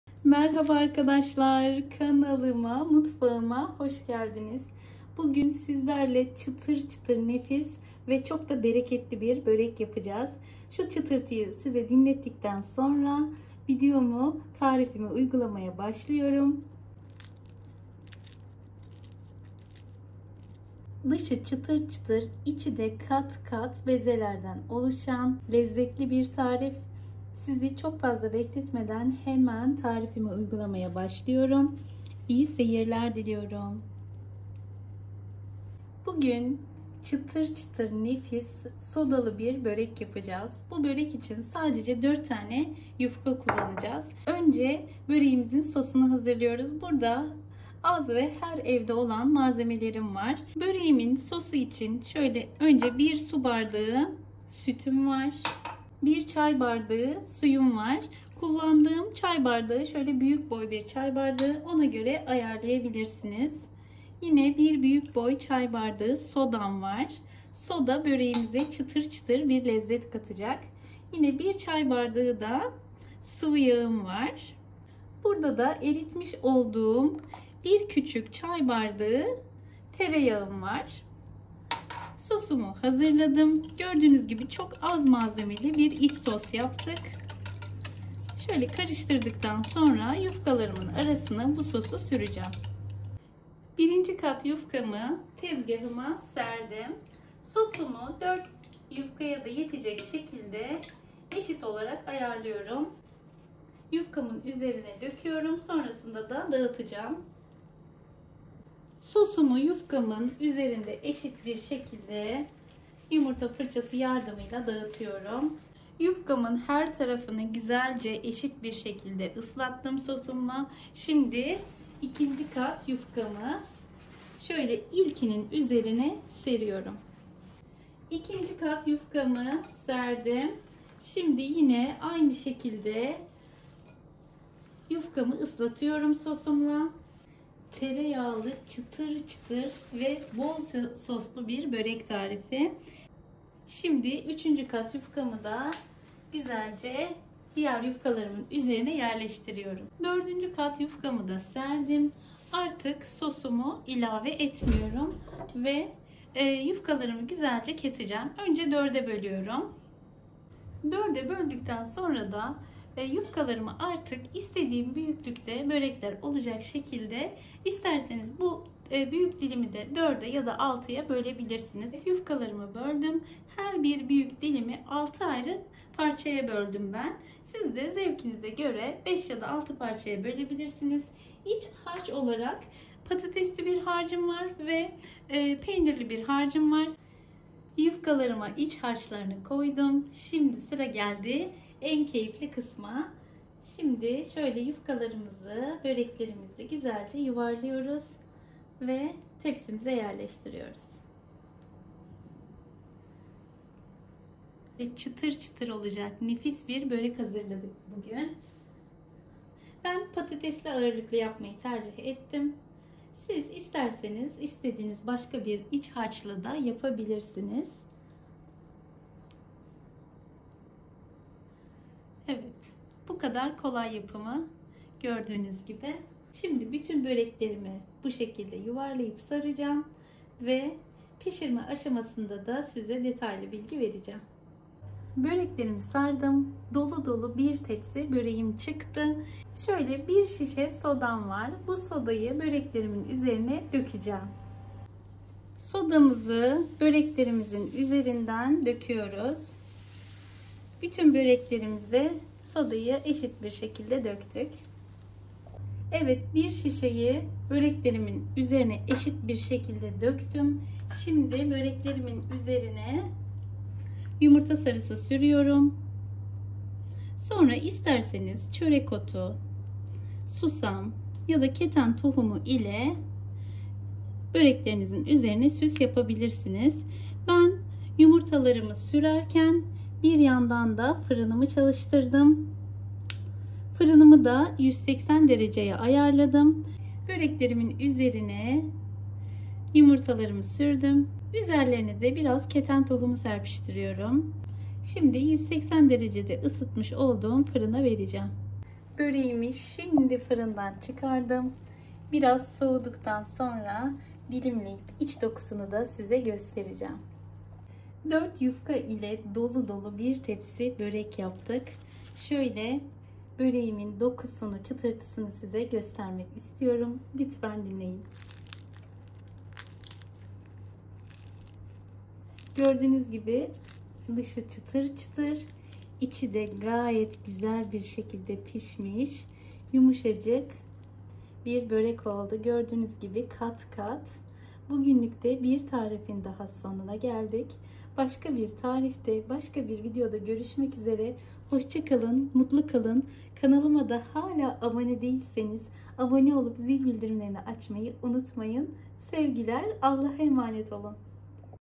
SODALI KATMER BÖREK TARİFİ ! LEZZET GARANTİLİ ÇITIR KATMER BÖREK TARİFİ /HAMURİŞİ TARİFLERİ
👉 Kolay Kahvaltı Video Serilerimizi İzlemeyi Unutmayın!

Çıtır çıtır sodalı katmer börek tarifi
* 4 tane yufka
Yufkaların arasına sürmek için sos
* 1 su bardaği süt
* 1 büyük çay bardağı sıvı yağ
* 1 büyük çay bardağı maden suyu
* 1 büyük çay bardağı su
* 1 küçük çay bardağı  eritilmiş tereyağı
Arasına:
* istediğiniz bir iç harç
Sosu için;
* 1 şişe Maden suyu
Üstüne:
* Yumurta sarısı
* Çörek otu, susam

KANALIMDA NELER VAR :)

SAMİMİ YORUMLARINI VE BEGENİLERİNİZİ BEKLİYORUM. SEVGİYLE KALIN